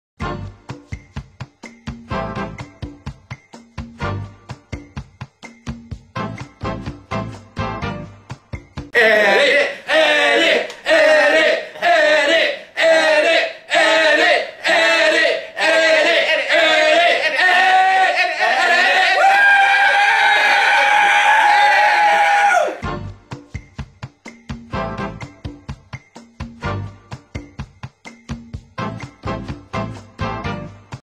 ECC Presents Motivational Techniqes
Ever wonder how ElectroCult Circus gets anything done? Us too? Well heres one way in a list of many that shows you how we get our video edited and ready for public consumption.